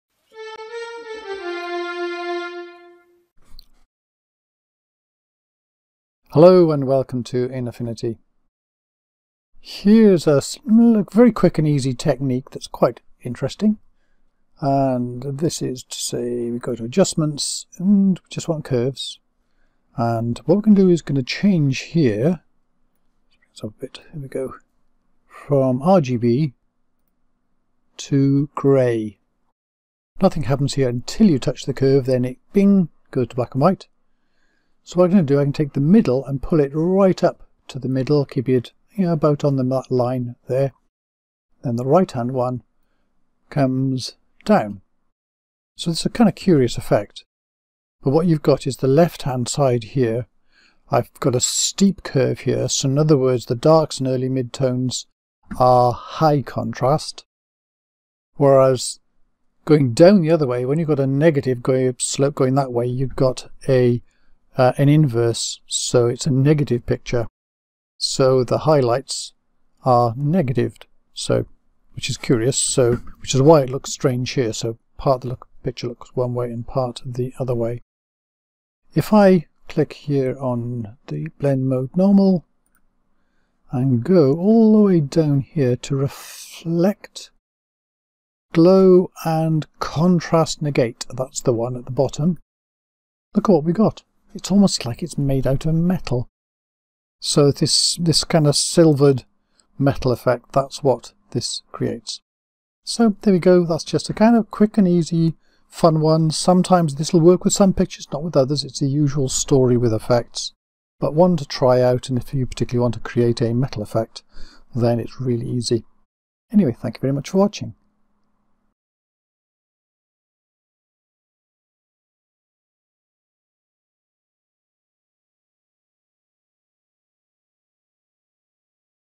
A Quick and Easy Silvered Effect using Affinity Photo
Here's a quick and easy way to create a silvered effect in a photo using the Contrast Negate blend mode in Affinity Photo (you won't find it in other editors).

The web page for this video is here (with links to related videos)